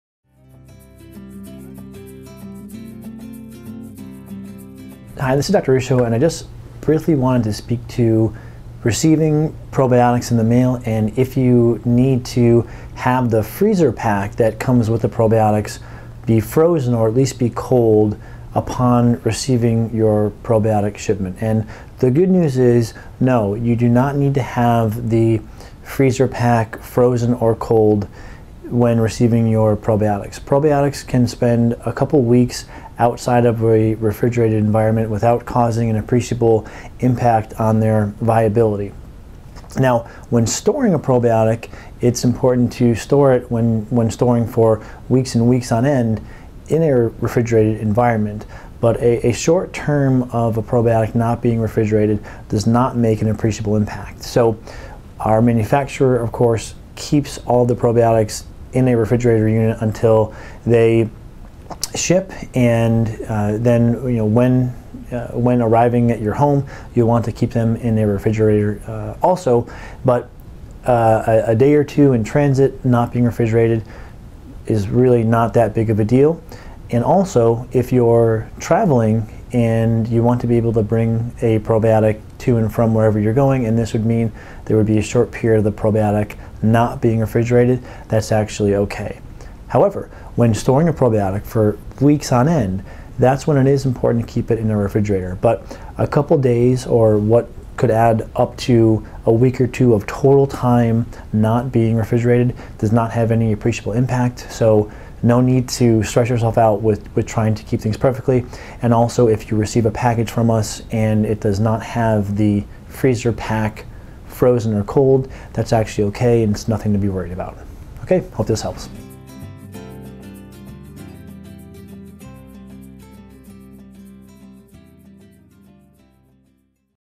Probiotics In The Mail
Do your probiotics need to ship cold and be stored cold?  In short, no. Want more info?